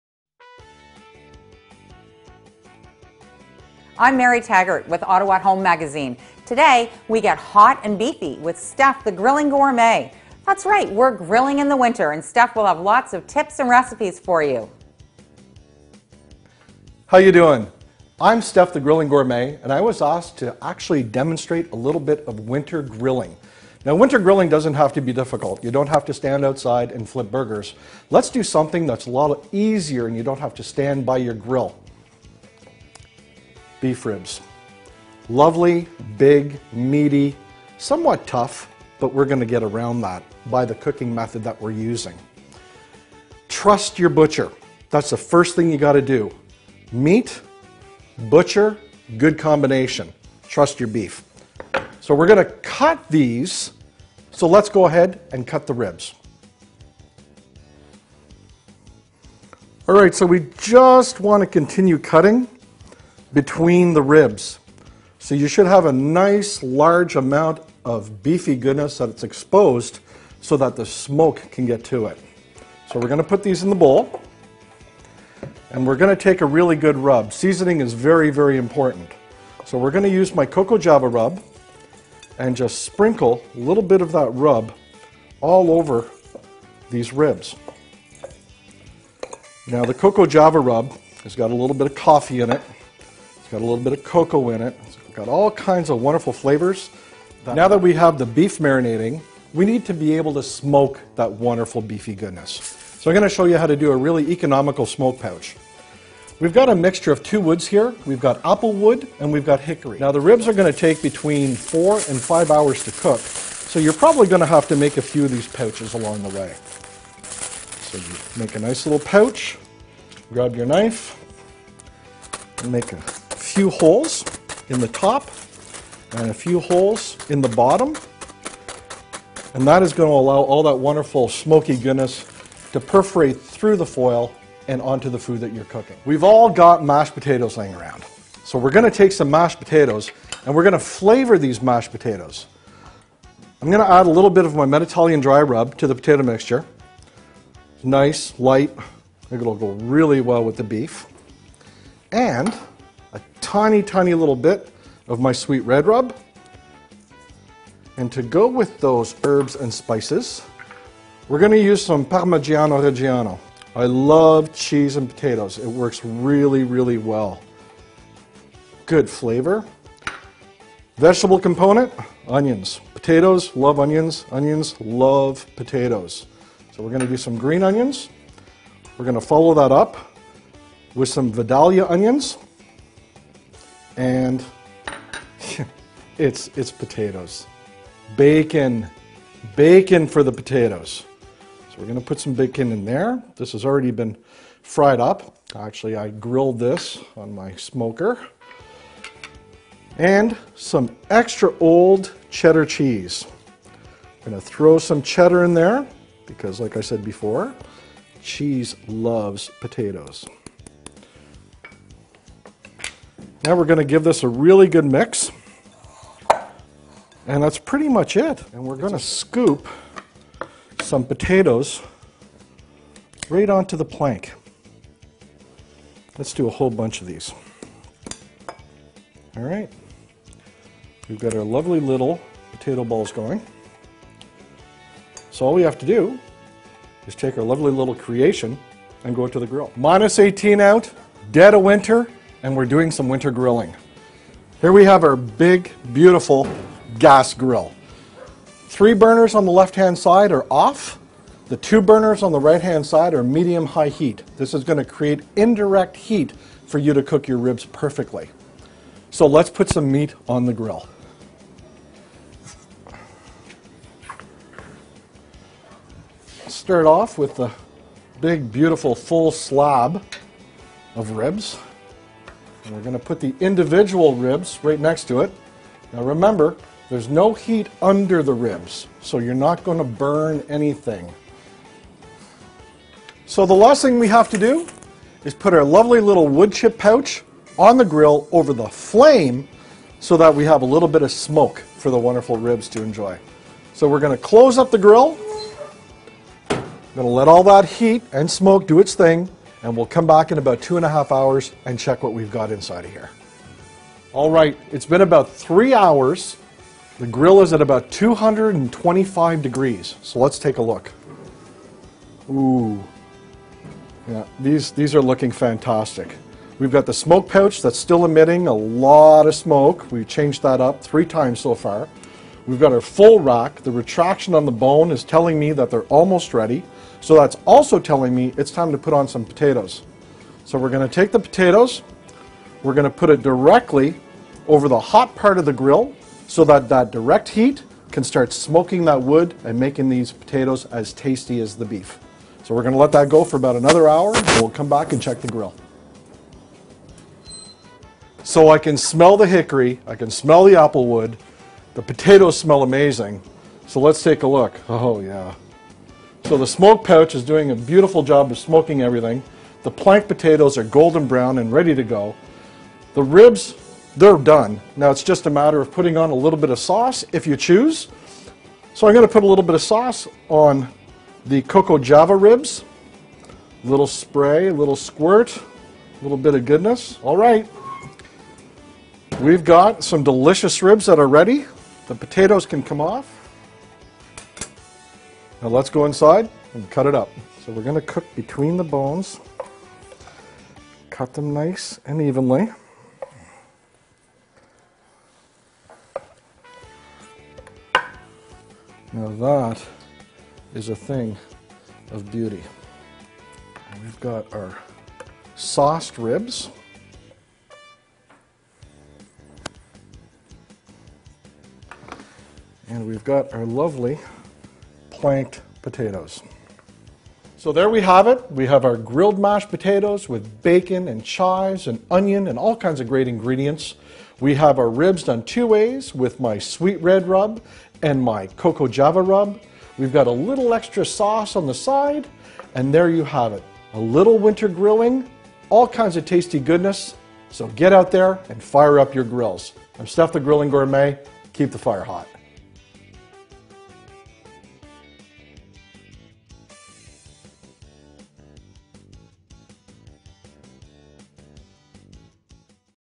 Winter Grilling with Ottawa At Home magazine and Steph Legari
Ottawa At Home magazine and Steph Legari in a winter grilling video. We grill beef ribs and potatoes to serve up a delicious hearty meal.